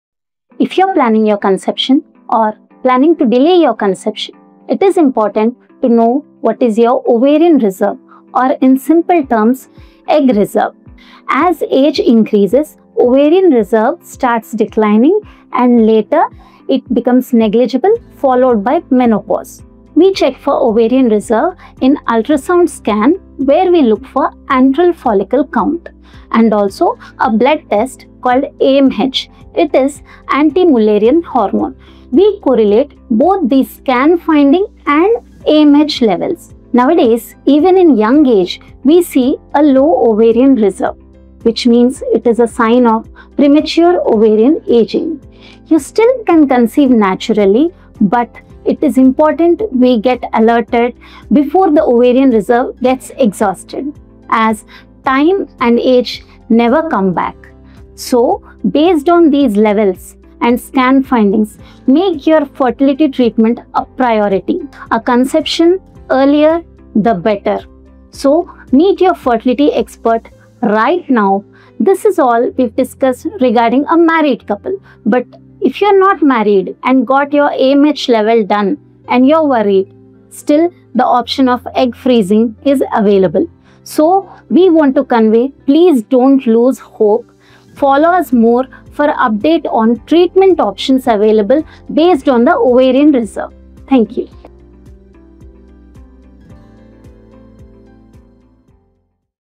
Know Your Ovarian Reserve (Egg Reserve) With Expert Consultation | Anu Test Tube Baby Centre
Know your Ovarian Reserve (Egg Reserve) with TRUE MED (where we also offer Consultation Services) & ANU Test Tube Baby Centre!

We use AMH levels and Ultrasound Scans to give you a clear fertility picture.

Don't let age or low reserve stop you—make fertility treatment a priority today!